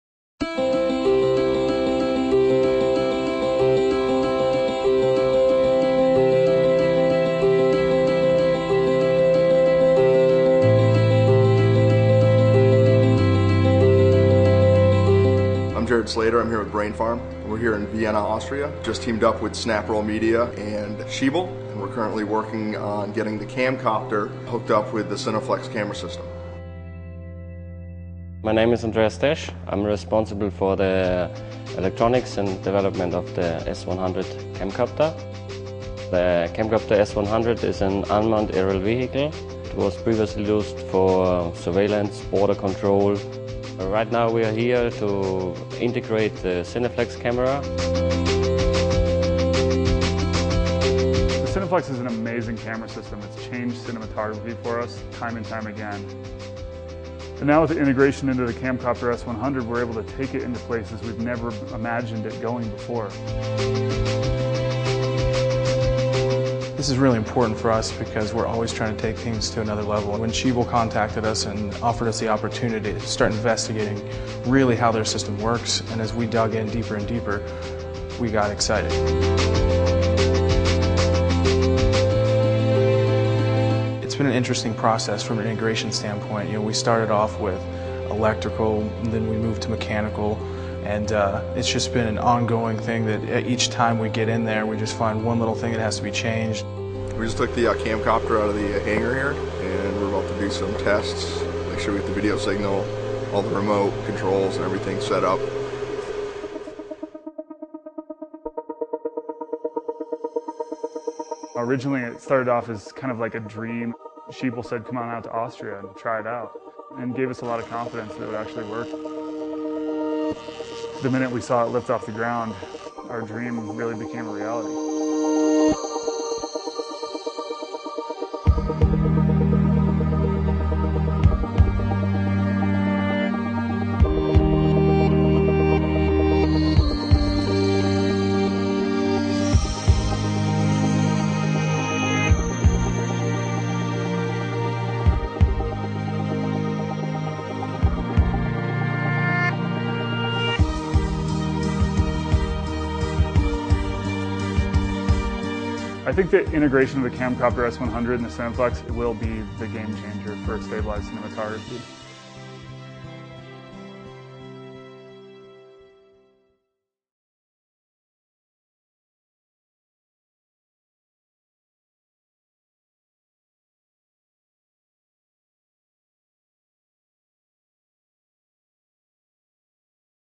The Schiebel CAMCOPTER® S-100 _ Cineflex Integration with Brain Farm and SnapRoll.flv
Schiebel's CAMCOPTER® S-100 UAS integrated with CINEFLEX® is the ultimate gyro-stabilized solution for aerial cinematography. The CINEFLEX® is a multi-axis coordinated gimbal control system that provides layered isolation, steering, and fine correctional movements to stabilize a wide selection of precision optics to a sub-pixel level. The S-100 navigates via pre-programmed GPS waypoints or is operated with a pilot control unit. Missions are planned and controlled via a simple point-and-click graphical user interface. The pair of these two high advanced systems gives us the ability to capture fully stabilized high quality images with ease.